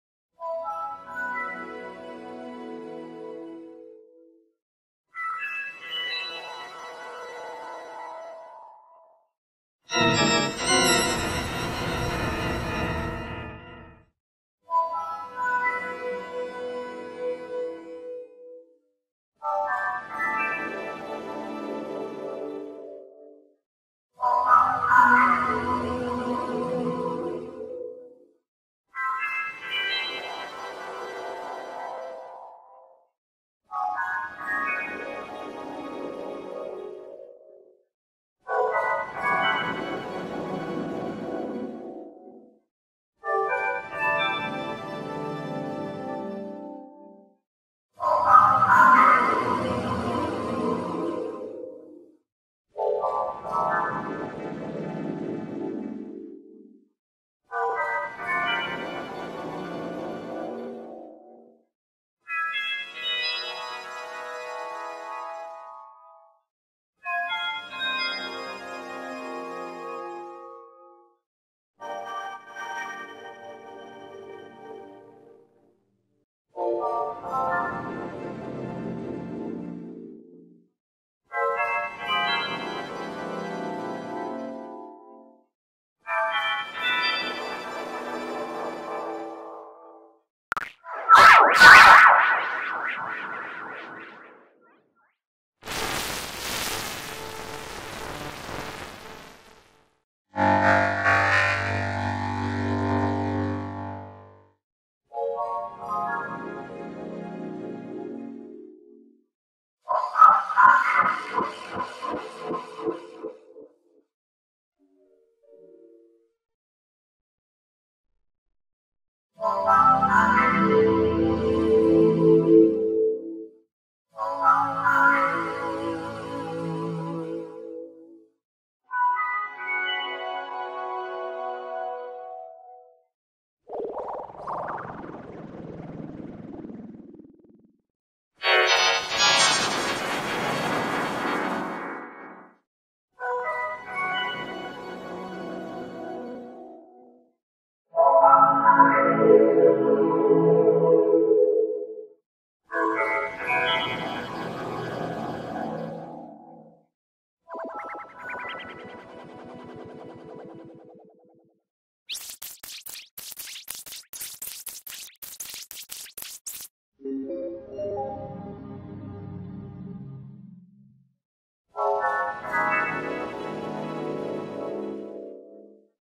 Windows Vista Effects 6 (My Sixth Preview)
Made today on ‎‎Sunday, ‎January ‎19, ‎2020, ‏‎12:13:33 pm to 12:15:31 pm

Here is a compilation of Windows Vista effects in my sixth preview.